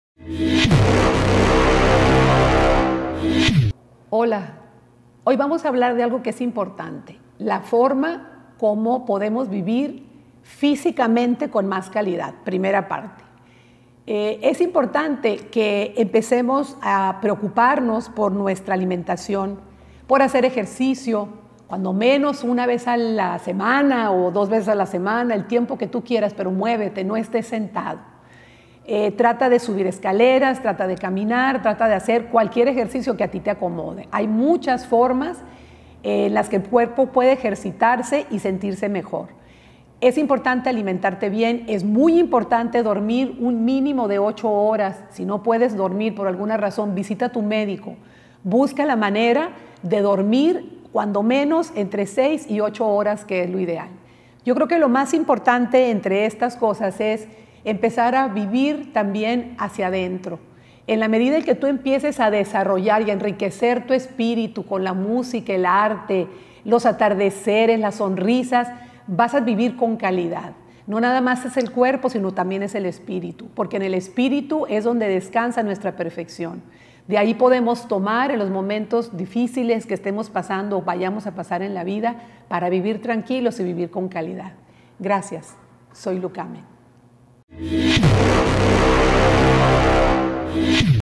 Vivir sanamente 1ª parte
Hacer ejercicio algunas veces por semana, una alimentación balanceada y un descanso apropiado son claves para vivir sanamente.